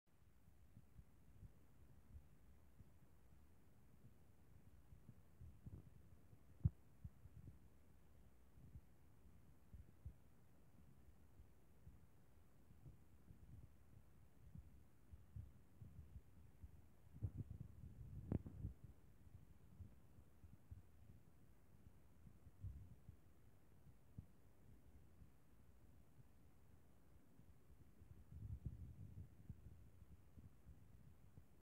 Mr. Cardinal as seen out the window on January 25, 2020
Our best winter bird friend stays close to the house! 😁